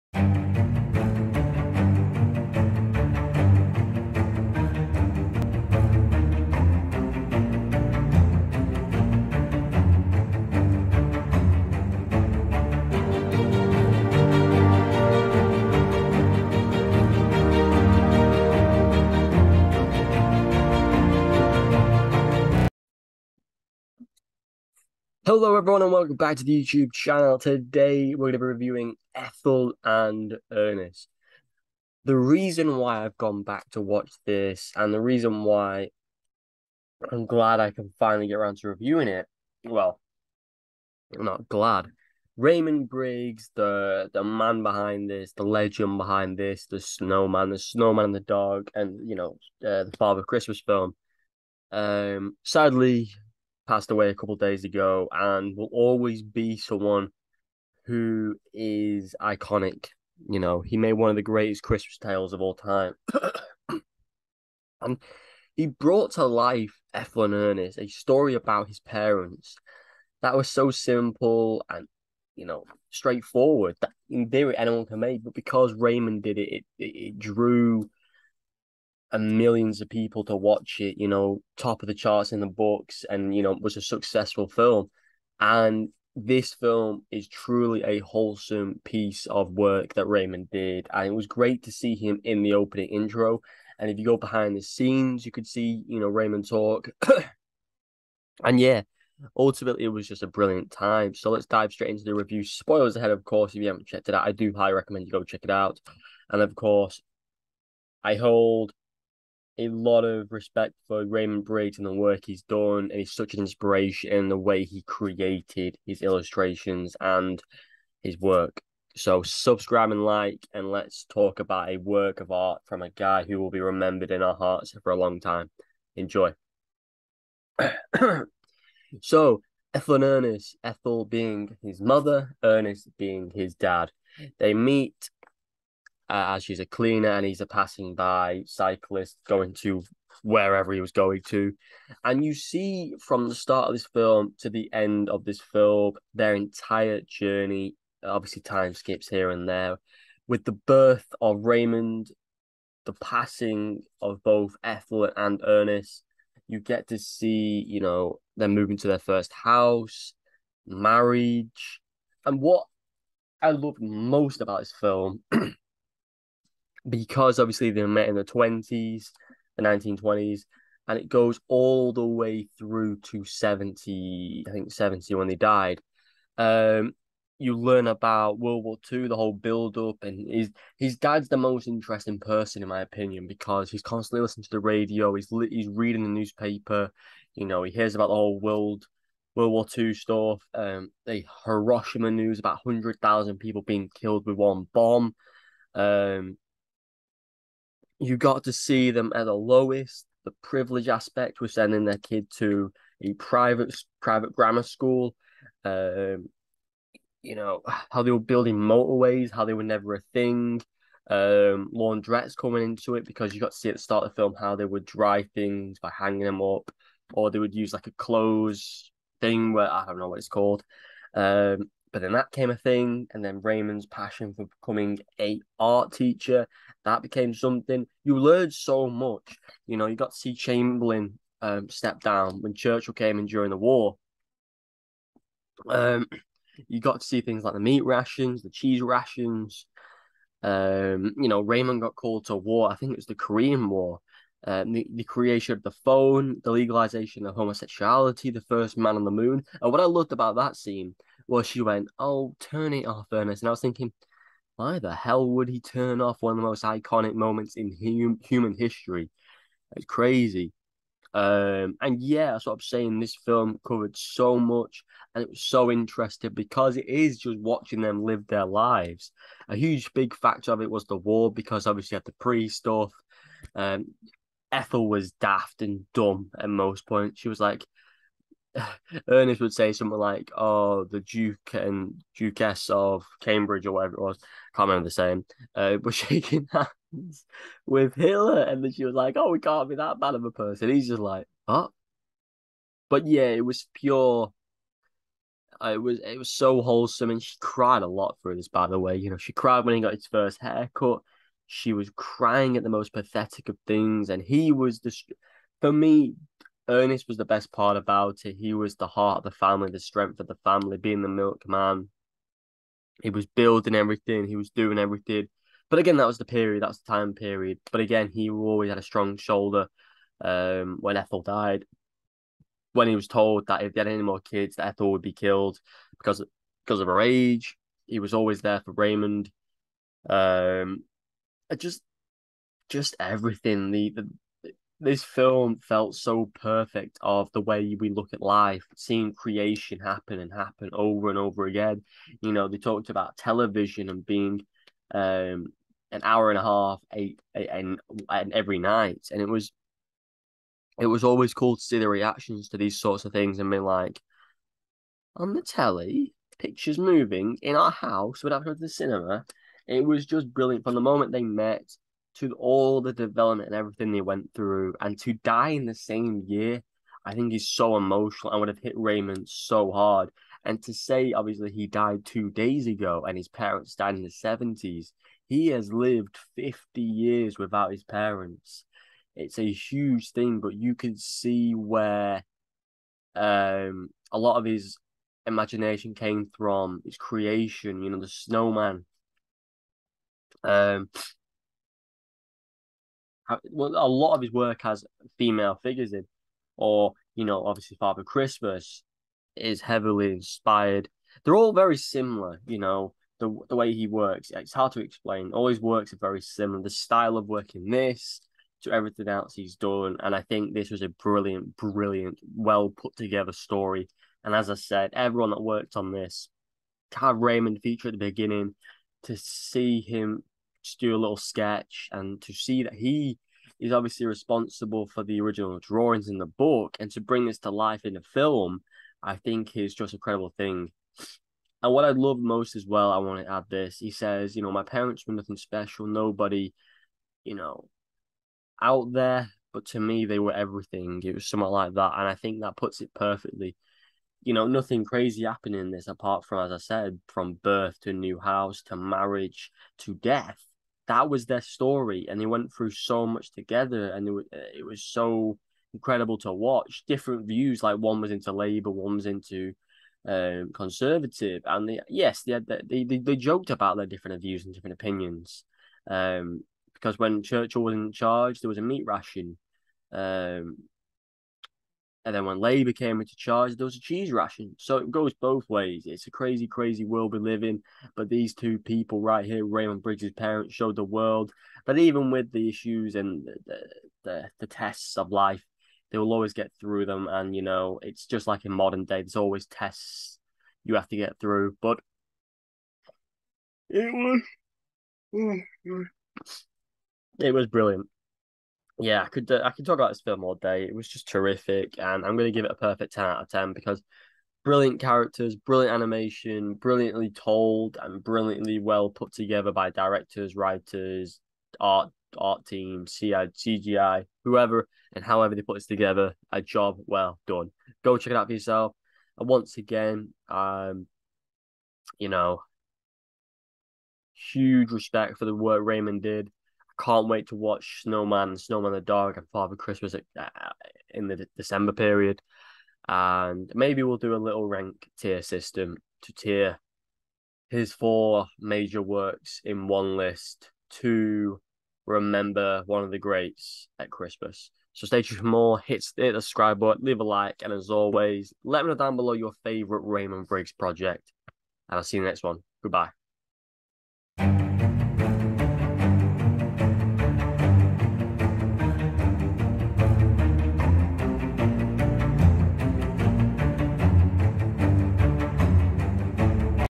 Ethel and Ernest Review
Today I will be reviewing Ethel and Ernest, including spoiler talk. If you have anything you would like to discuss feel free to leave a comment down below and if there is any movies you wanna see me review feel free to let me know. anything else you wanna see.